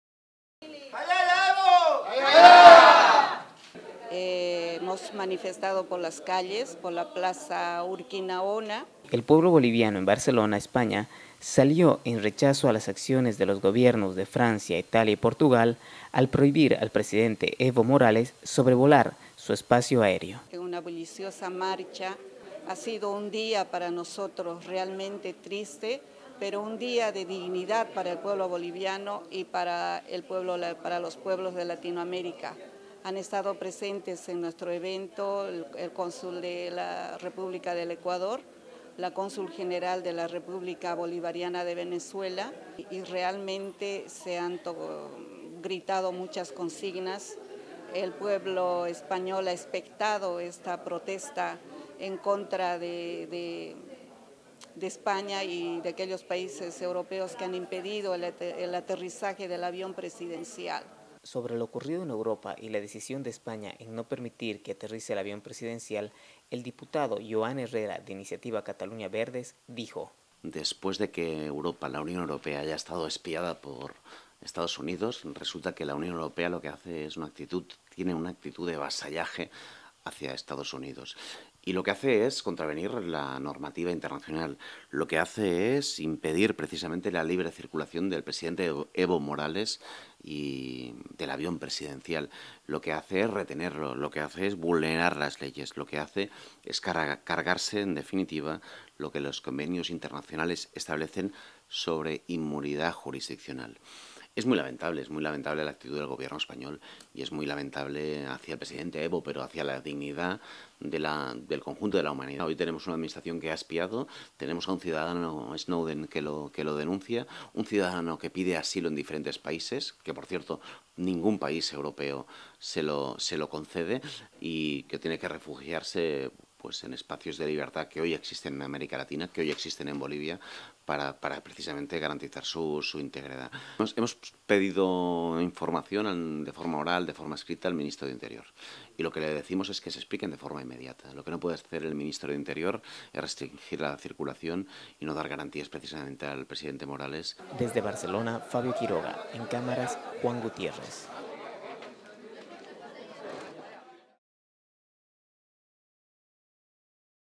Bolivianos en Barcelona manifiestan su indignación y apoyo al Pdte. Evo Morales,
Bolivianos en Barcelona manifiestan su indignacion y portestan ante los echos sucedidos al presidente evo morales.
Entrevista a la Consul de Bolivia en Barcelona: Alicia Muñoz Ala
Entrevista al Diputado Joan Herrera de Iniciativa Cataluña Verdes -ICV: